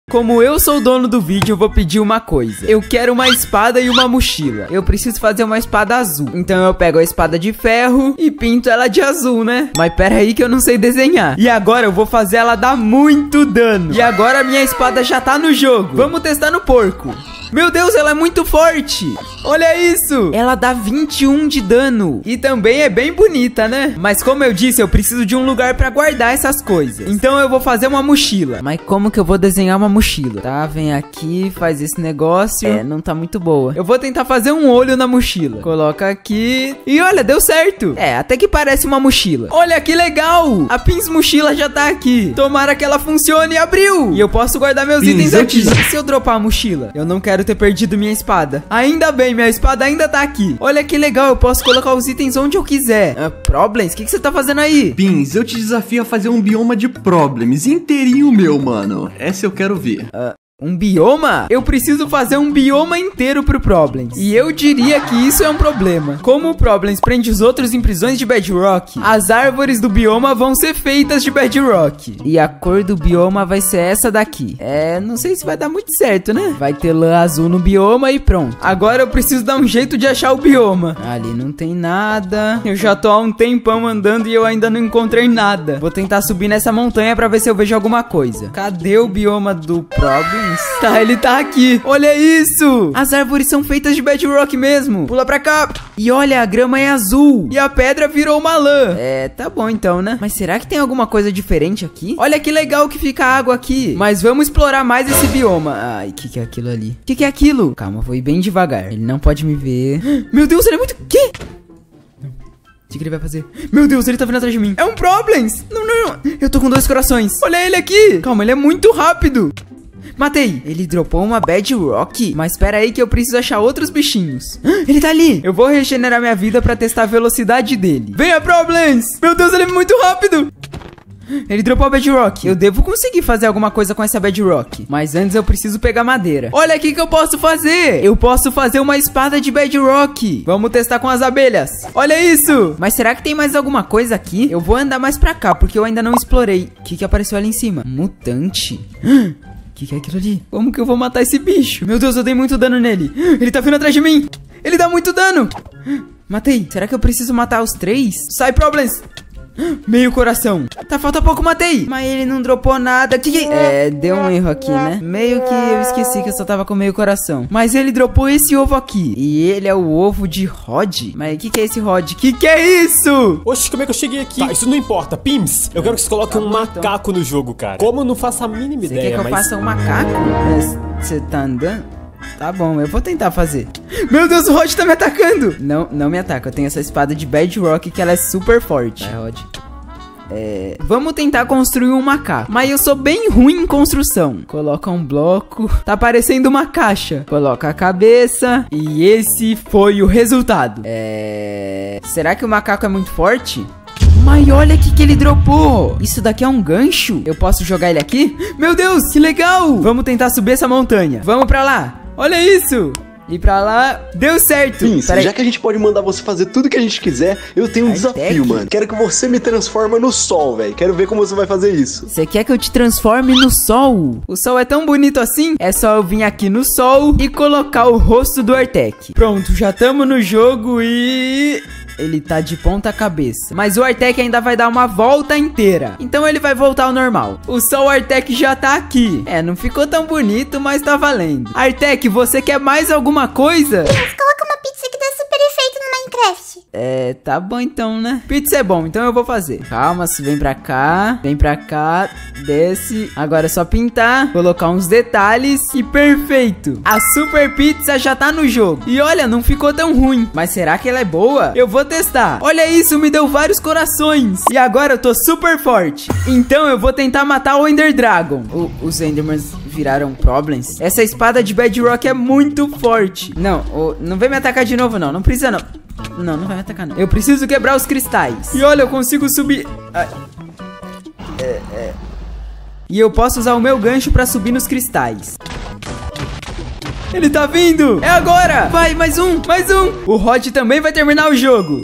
Os Youtubers ENTRARAM E CONTROLARAM MEU JOGO
Nesse vídeo eu Fiz Tudo Que Os Youtubers Mandaram! Eu tive que criar um novo bioma de problems para o jogo!